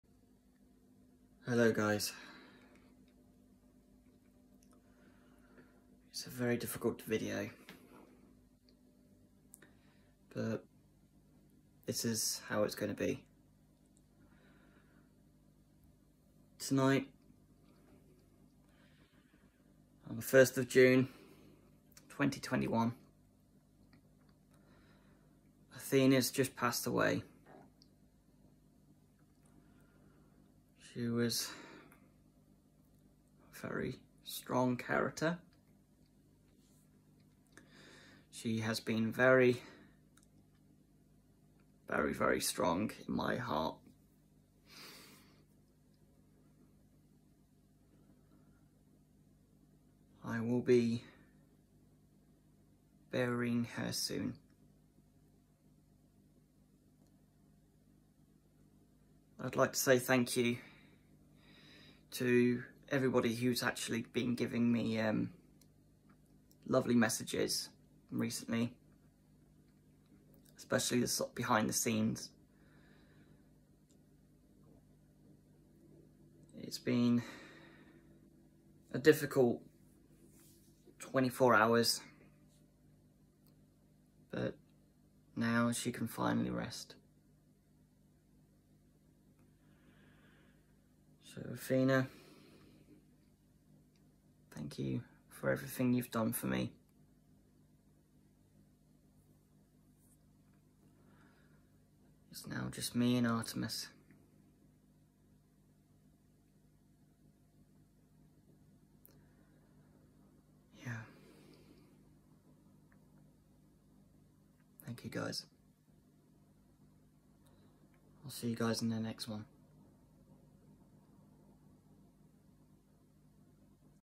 Got some news.
Rip Athena, we will all miss you dearly. Sleep well sweetheart. Good bye old friend.